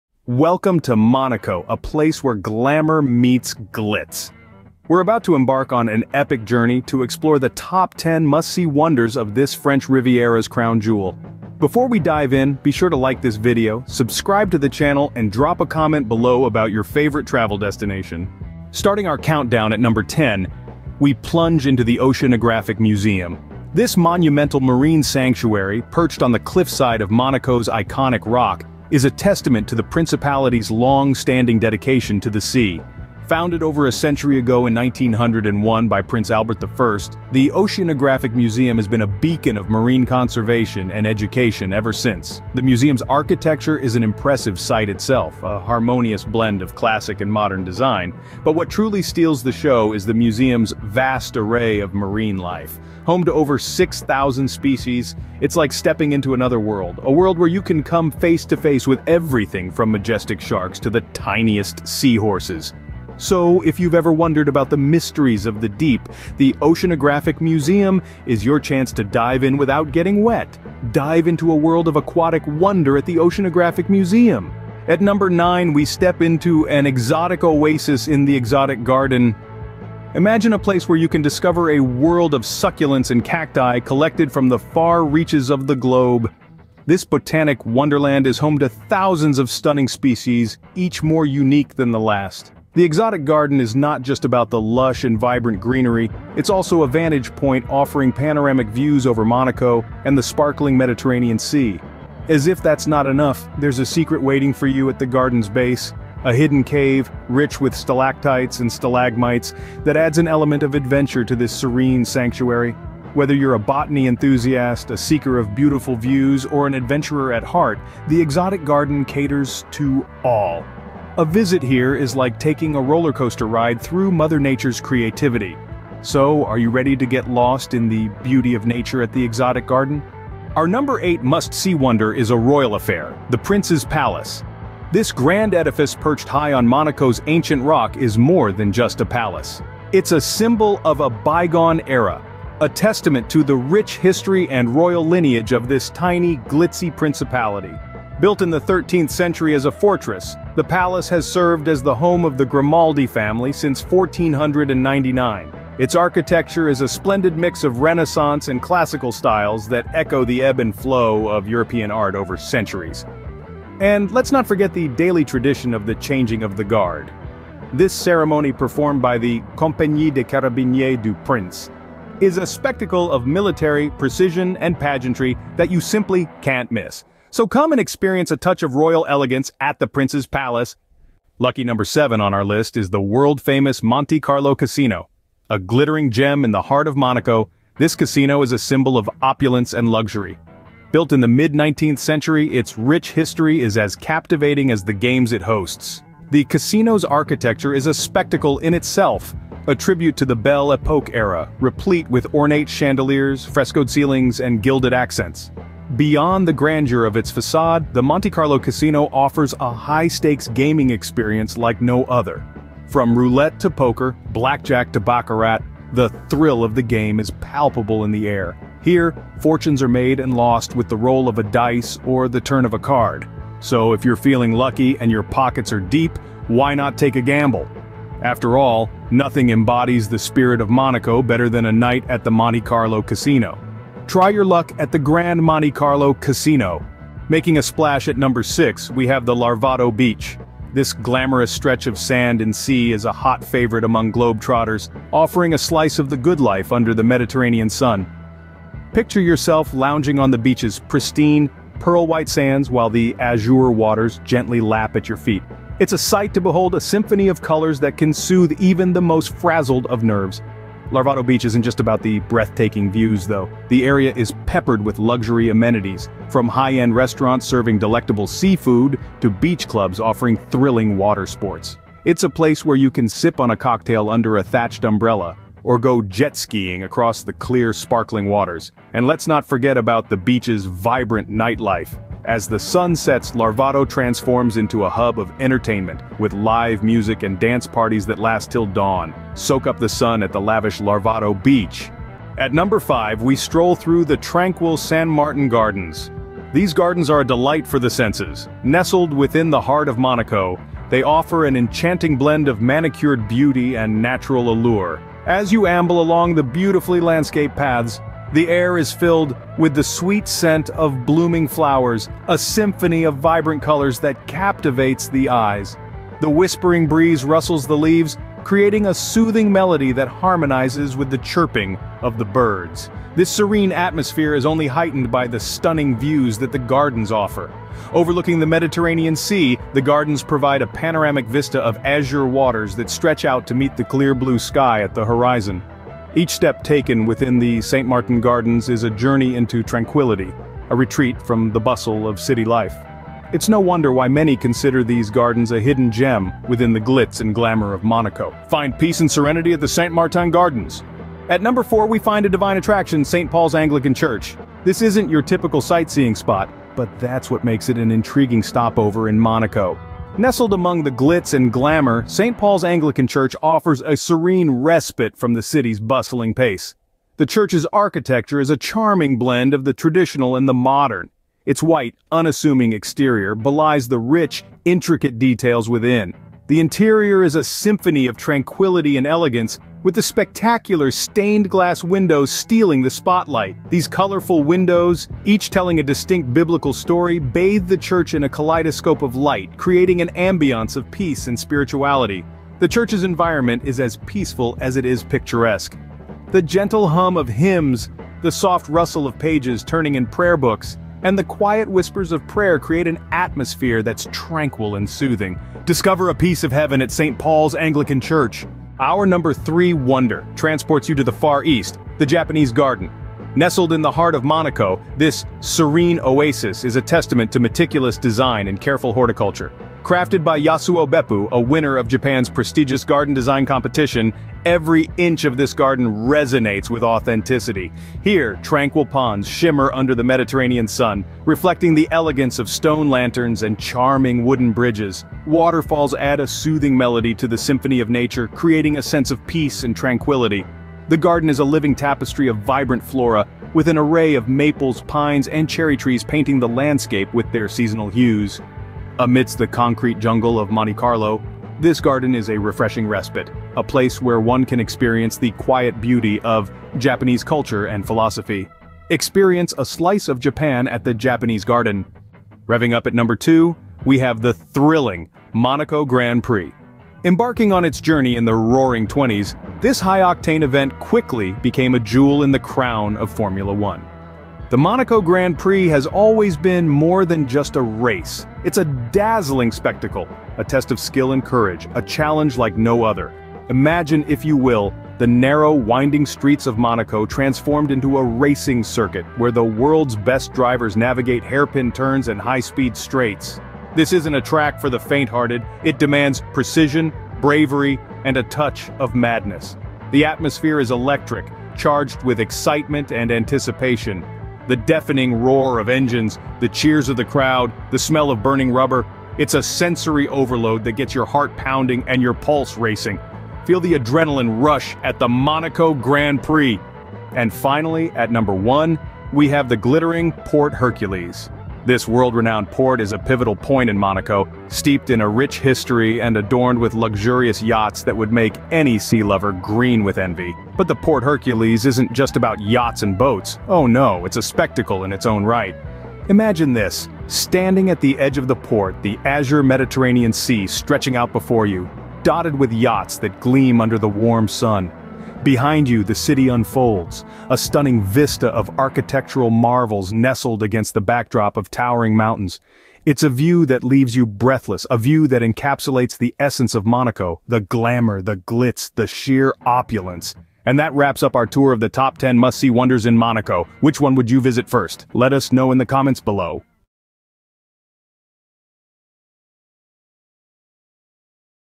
Top 10 Must-See Wonders in Monaco! The Glamour and Glitz of the French Riviera's Crown Jewel!
🌟 Welcome, fellow travelers, to a world of opulence and allure as we present "Top 10 Must-See Wonders in Monaco!" 🇲🇨 Embark on a journey through the glamour and glitz of the French Riviera's crown jewel.

✨ The Grand Opening at 00:00:00 sets the stage for an exploration like no other, where each wonder unfolds with elegance and sophistication.

🐠 Dive into the depths of wonder at "The Oceanographic Museum" at 00:00:18, where marine marvels captivate the senses in an architectural masterpiece overlooking the Mediterranean.

🌵 Explore the enchanting terraces of "The Exotic Garden" at 00:01:14, a botanical gem offering breathtaking views of Monaco's breathtaking landscapes.

🏰 Ascend to regal heights at "The Prince's Palace" at 00:02:10, where centuries of history and royal legacy echo through the halls of this iconic residence.

🎲 Roll the dice in the glitzy world of "Monte Carlo Casino" at 00:03:01, a playground for the elite where fortunes are made and lost against the backdrop of luxury.

🏖️ Bask in the sun-drenched allure of "The Larvotto Beach" at 00:03:57, where golden sands meet the azure sea, inviting you to relax in Monaco's coastal splendor.

🌺 Stroll through the botanical wonders of "Saint-Martin Gardens" at 00:04:52, an oasis of tranquility nestled in the heart of the city.

⛪ Discover spiritual serenity at "Saint Paul's Anglican Church" at 00:05:51, a historic sanctuary standing as a testament to Monaco's diverse cultural tapestry.

🌿 Find Zen in "The Japanese Garden" at 00:06:48, a harmonious retreat where Eastern aesthetics merge with the Mediterranean landscape.

🏎️ Feel the pulse of speed at "The Monaco Grand Prix" at 00:07:42, a legendary street circuit that roars to life with the adrenaline of motorsport enthusiasts.

🔔 As the curtains draw close with "The Wrap Up" at 00:08:36, you'll be left with memories of Monaco's unparalleled wonders. Let the adventure unfold!